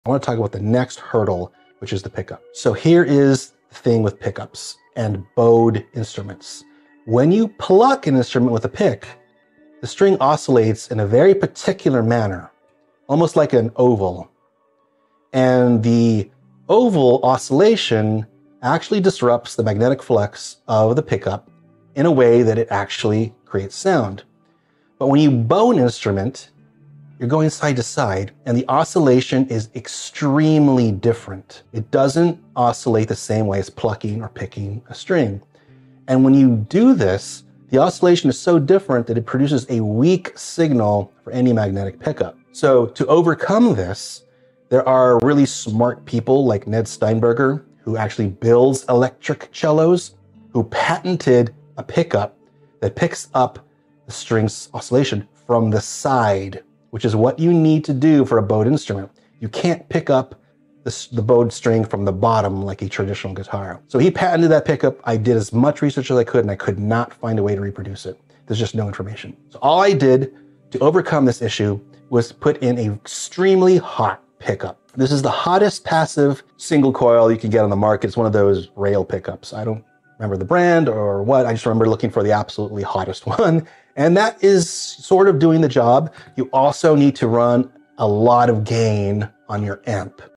Pickup Problems: How Bowed Instruments Challenge Sound
We dive into the challenge of pickups for bowed instruments. Discover how oval string oscillations affect magnetic flux and why standard pickups struggle. Learn about Ned Steinberger's innovative solution and our approach to overcoming this issue using a hot pickup and amp gain.